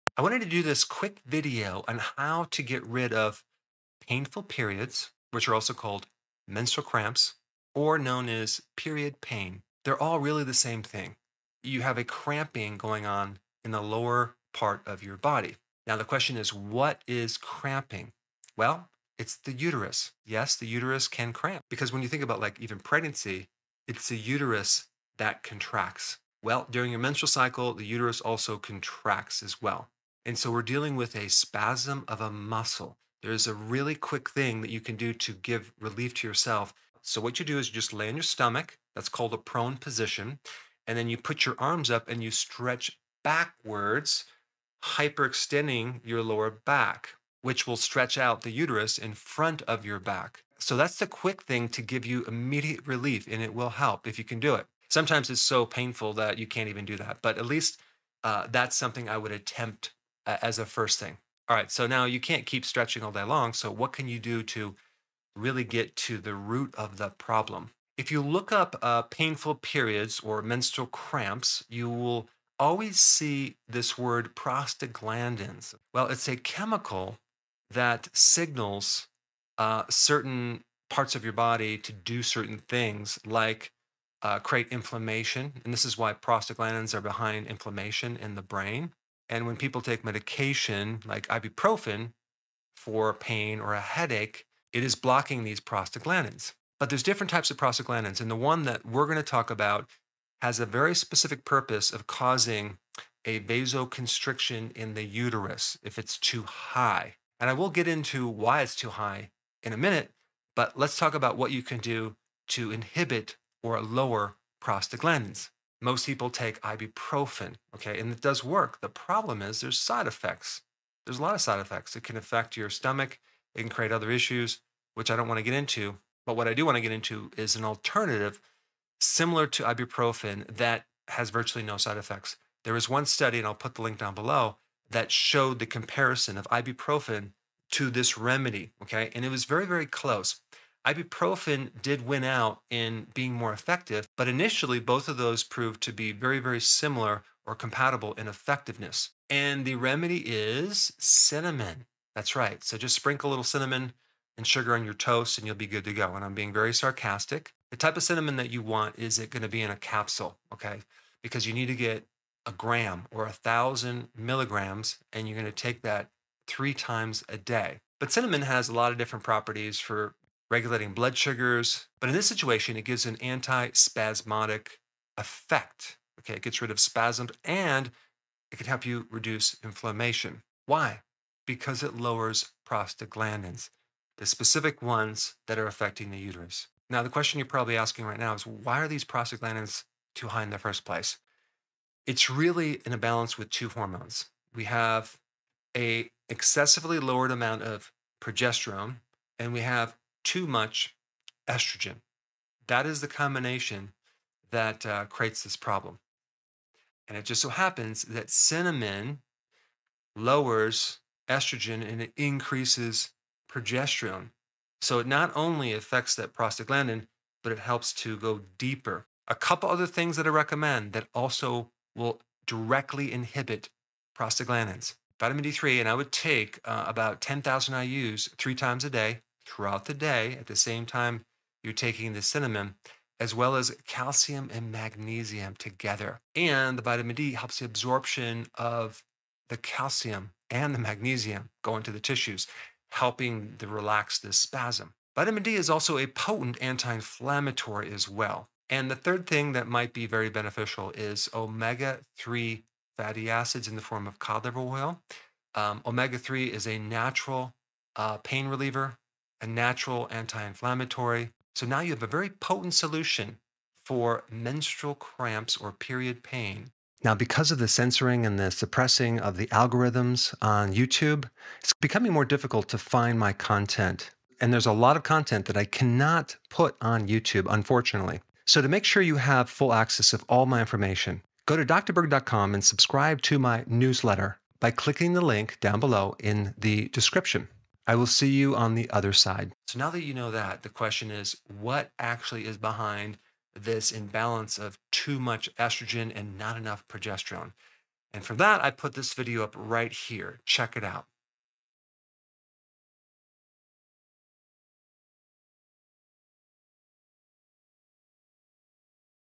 How to INSTANTLY Relieve Painful Periods (Menstrual Cramps)
Taking ibuprofen can lead to a myriad of side effects. Discover the best home remedies for period cramps.

0:00 Introduction: How to relieve painful periods
0:10 What causes period cramps?
0:30 The best stretch for menstrual cramps
1:00 Menstrual cramps explained
1:50 How to reduce menstrual pain naturally
4:27 Make sure you can always find unfiltered health information by signing up for my newsletter!
4:49 Learn more about estrogen!

Let’s talk about how to stop period pain or menstrual cramps.

With period cramps, you experience cramping in the lower part of the body. It’s the uterus itself that’s cramping or contracting, which can cause a painful period.

A great way to get instant relief from menstrual cramps is to try stretching. To do this stretch, you need to lay on your stomach and push your hands into the ground straightening your arms and stretching your back.

However, to get to the root cause of the issue and get rid of menstrual cramps, we need to lower a chemical in the body called prostaglandins.

People will typically use ibuprofen to lower prostaglandins. But ibuprofen has side effects. Thankfully, there are natural alternatives you can try to help lower prostaglandins.

The best natural remedies for menstrual cramps:
1. Cinnamon
2. Vitamin D3, calcium, and magnesium
3. Omega-3 fatty acids (cod liver oil)